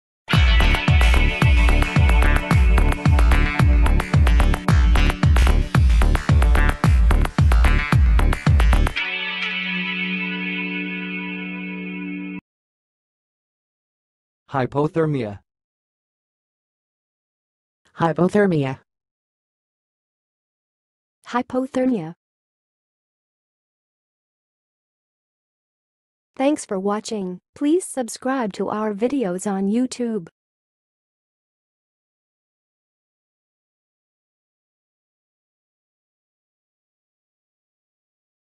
How to say "hypothermia"! (High Quality Voices)
Watch in this video how to say and pronounce "hypothermia"!